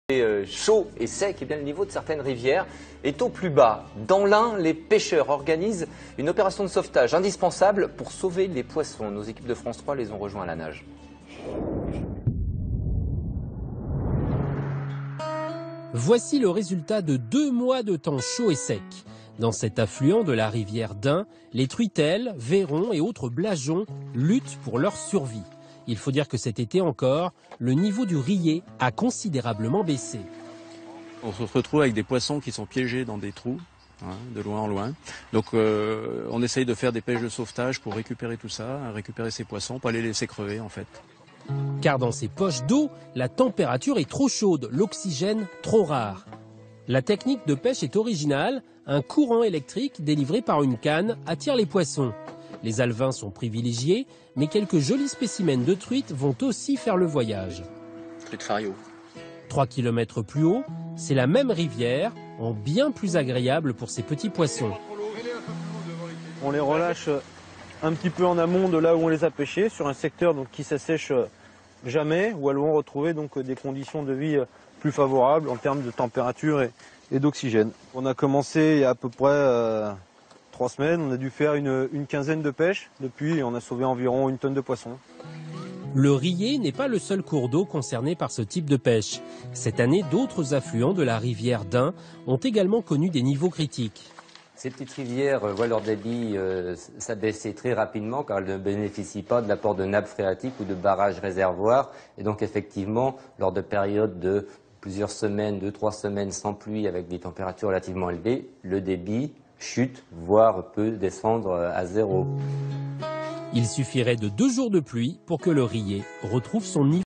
Les poissons menacés d'asphyxie 23 septembre 2013
Après notre été chaud et sec, le niveau de certaines rivières est au plus bas... dans l'Ain, les pêcheurs organisent une opération de sauvetage indispensable pour sauver les poissons